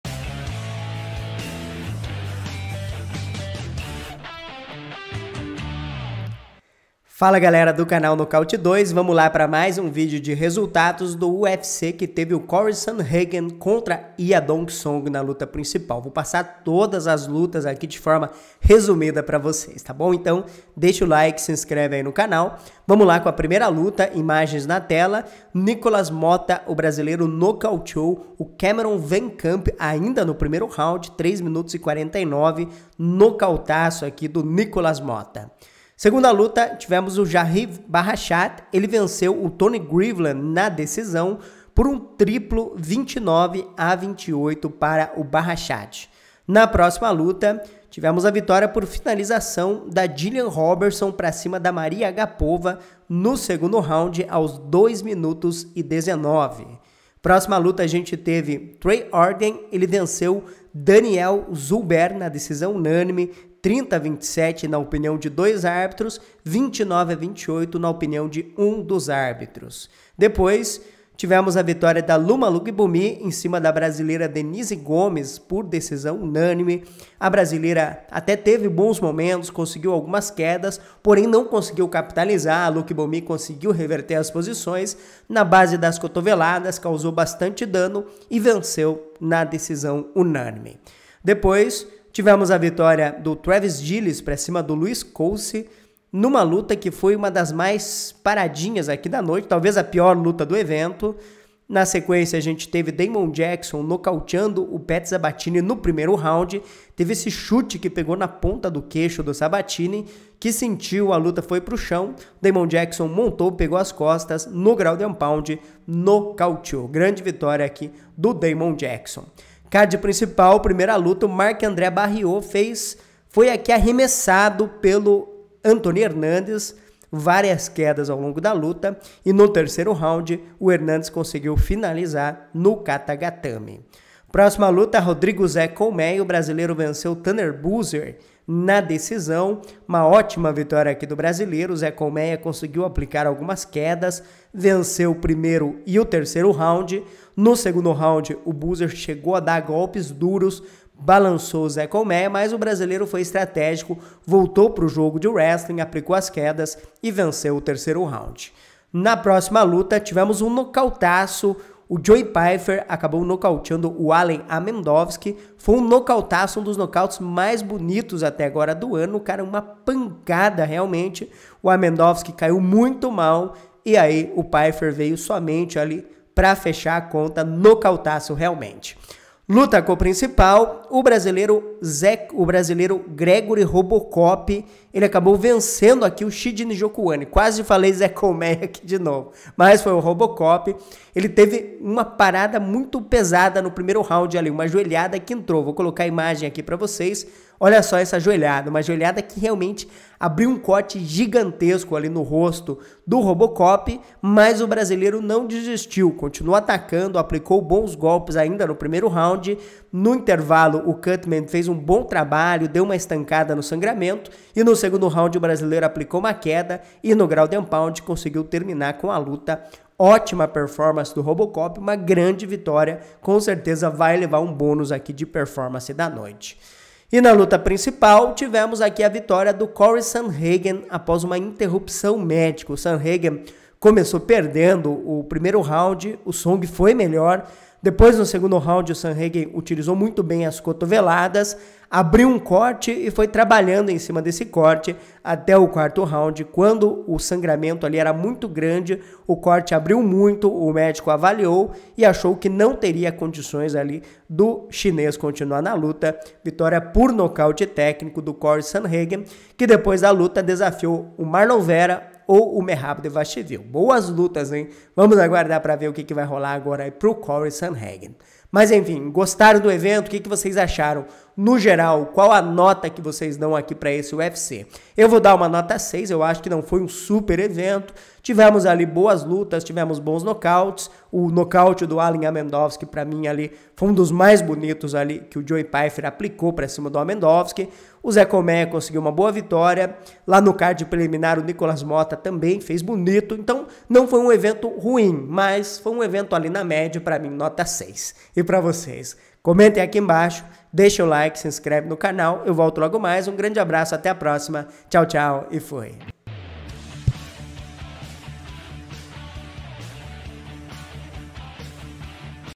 TODOS OS RESULTADOS DO UFC SANDHAGEN vs. SONG
UFC Sandhagen x Song

Cory Sandhagen venceu Song Yadong por nocaute técnico (interrupção médica) aos 5min do R4
Gregory Rodrigues venceu Chidi Njokuani por nocaute técnico no R2
Andre Fili venceu Bill Algeo por decisão dividida (29-28, 28-29 e 29-28)
Joe Pyfer venceu Alen Amedovski por nocaute técnico no R1
Rodrigo Zé Colméia venceu Tanner Boser por decisão dividida (30-27, 28-29 e 29-28)
Anthony Hernandez venceu Marc-André Barriault por finalização no R3
CARD PRELIMINAR:
Damon Jackson venceu Pat Sabatini por nocaute técnico no R1
Trevin Giles venceu Louis Cosce por decisão unânime (30-27, 29-28 e 29-28)
Loma Lookboonmee venceu Denise Gomes por decisão unânime (triplo 29-28)
Trey Ogden venceu Daniel Zellhuber por decisão unânime (30-27, 30-27 e 29-28)
Gillian Robertson venceu Mariya Agapova por finalização aos 2min19s do R2
Javid Basharat venceu Tony Gravely por decisão unânime (triplo 29-28)
Nikolas Motta venceu Cameron VanCamp por nocaute técnico no R1